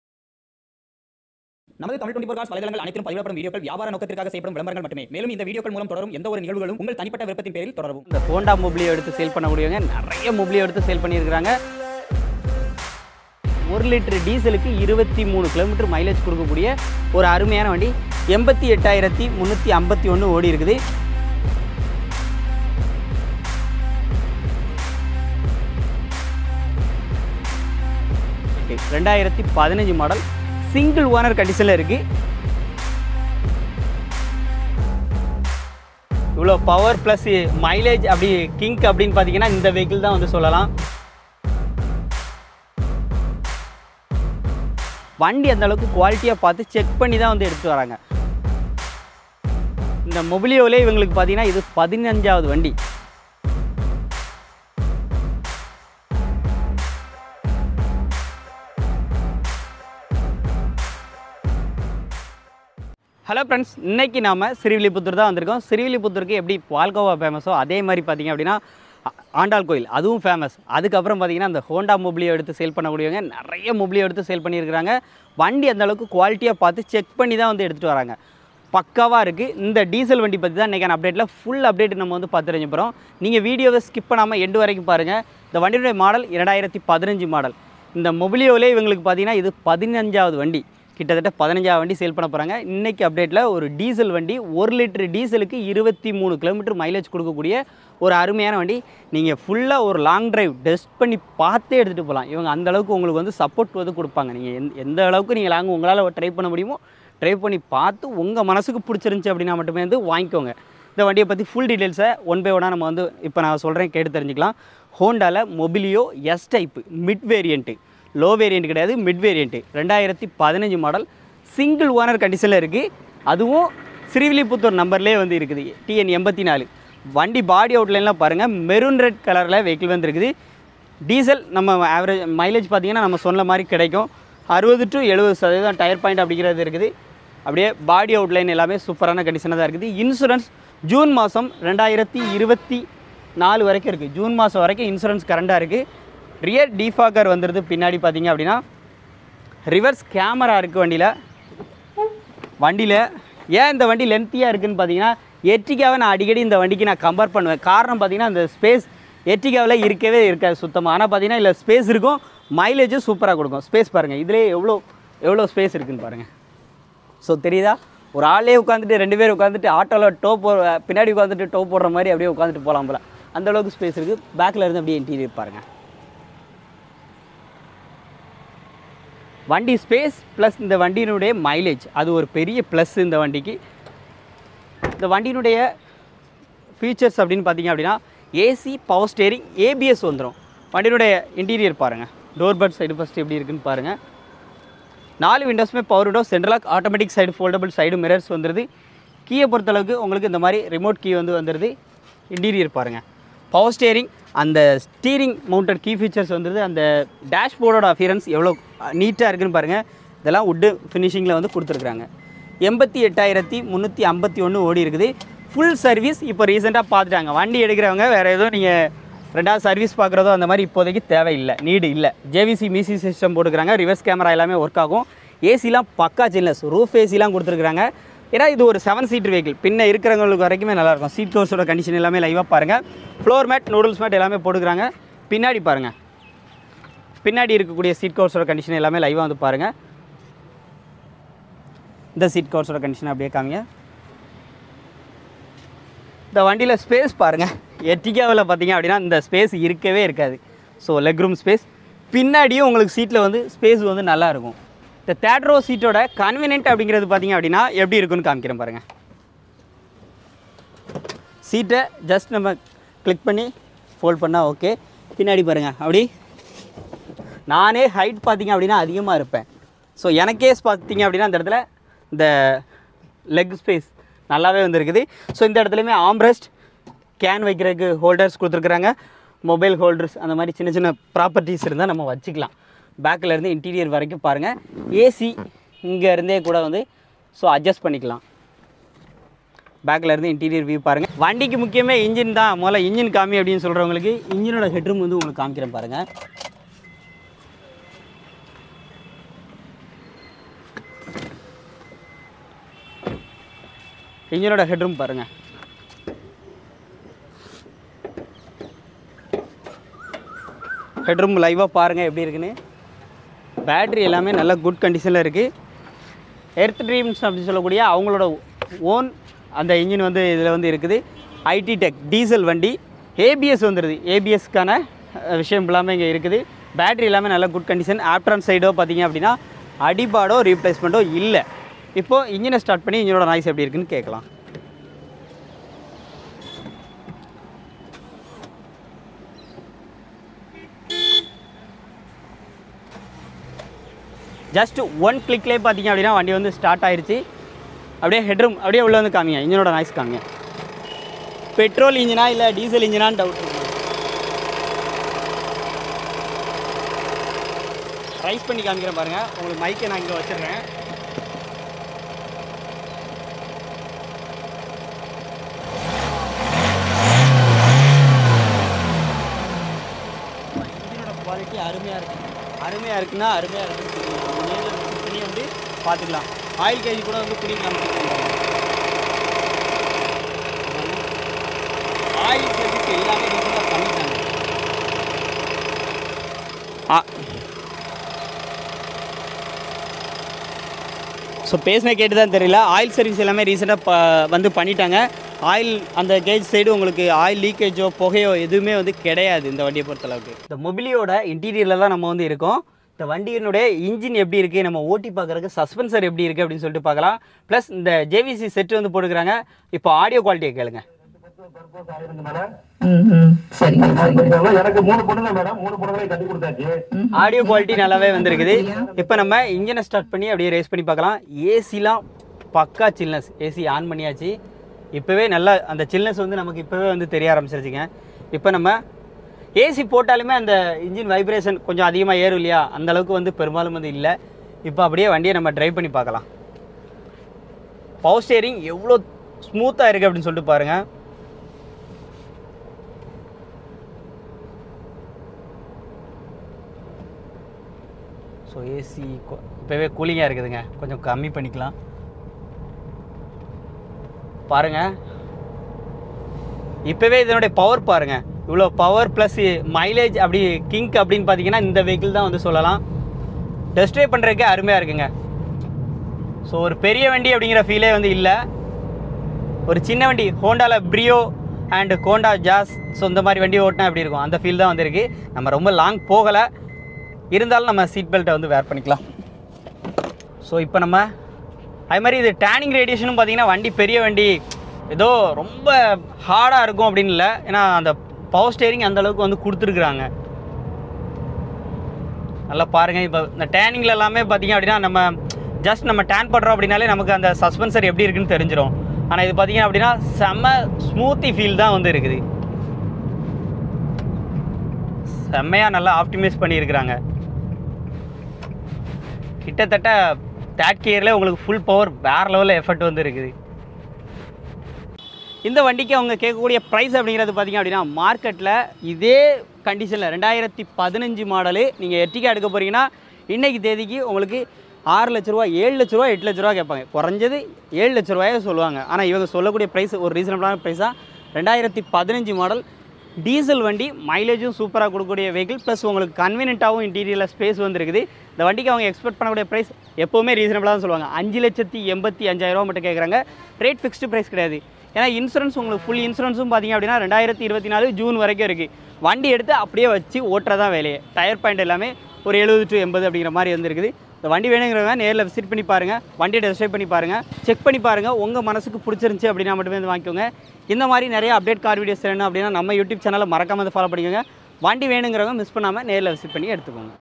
Ertiga இனி தேவையில்லை Mobilio போதும் | தமிழ் 24/7
Sold Out
Check The New Update Videos

Location : Srivilliputtur
Honda Mobilio 2015 1.5 S I DTEC Diesel
7 seater vehicle
88000 kms
Single owner
Current Insurance till Jun 2024
Good Engine Condition with genuine kms
ABS Braking
JVC Touch screen music system
Reverse camera
Chill AC
Power window
Central lock
Leather seat covers
New floor mats
Good battery condition
Steering Mounted Audio control
Mileage city - 17 Km/ltr Highway - 24 Km/ltr
Full service done. No need to spend anything.
Looks like brand new vehicle.
compared to Ertiga This is very gud vehicle
Price : 5.85 lakhs(Slightly Negotiable)